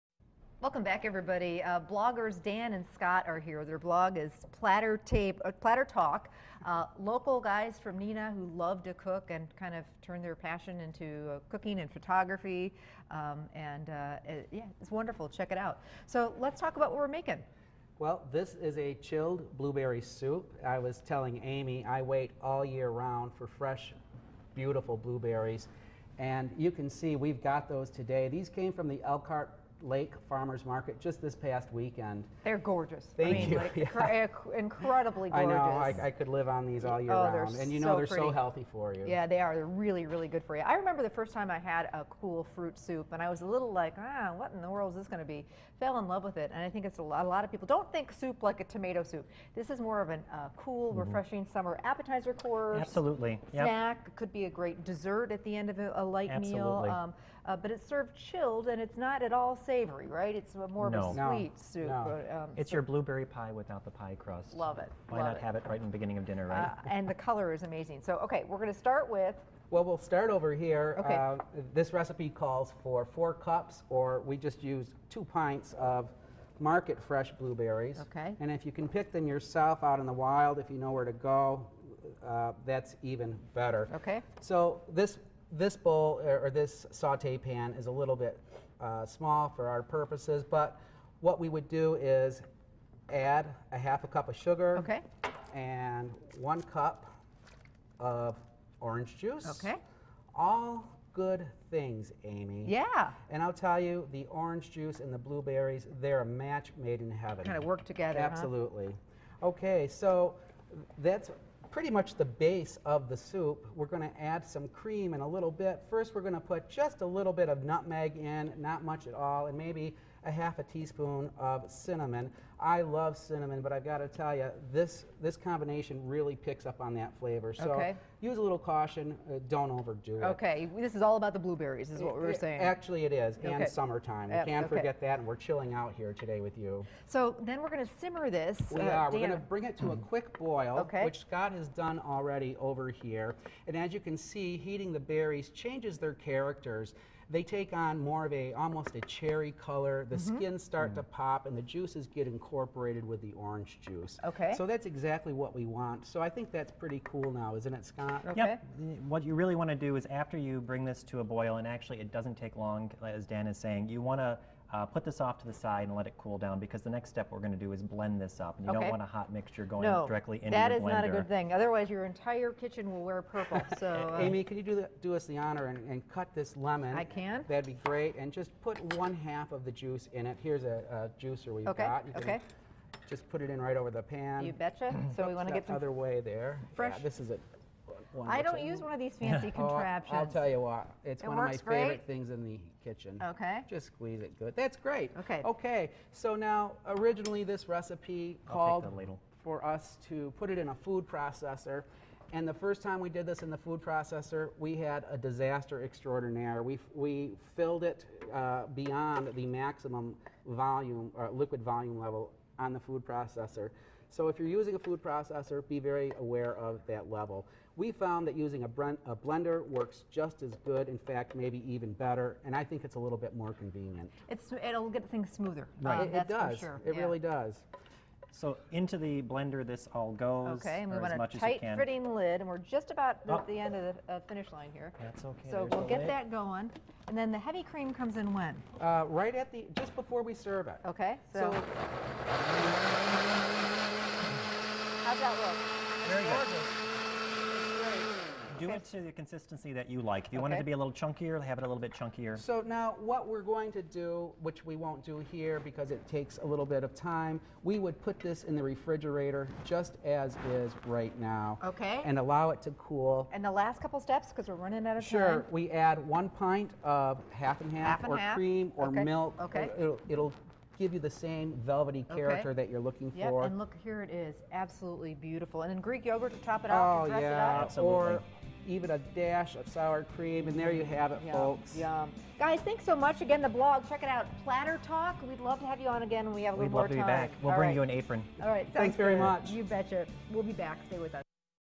Chilled Blueberry Soup
Light and refreshing cold soup recipe, perfect for summer.